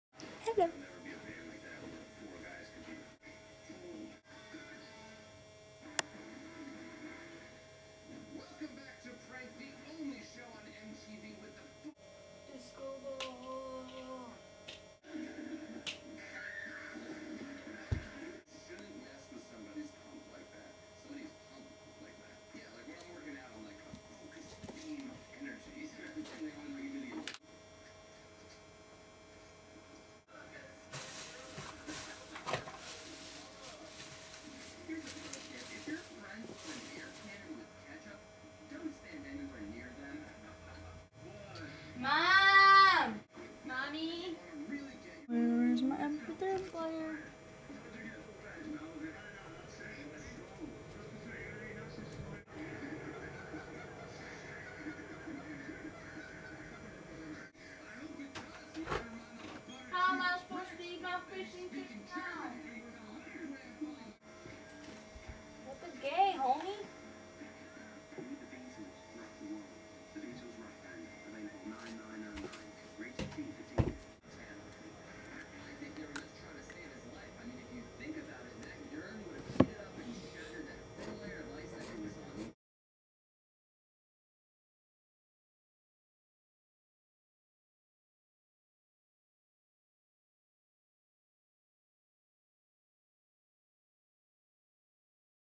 Mp3 Player Madness
I Was Gunna Make A Video, That Was Actually, Gunna Be Good. But, I Had No music..And I Couldn't Find My Mp3 Player):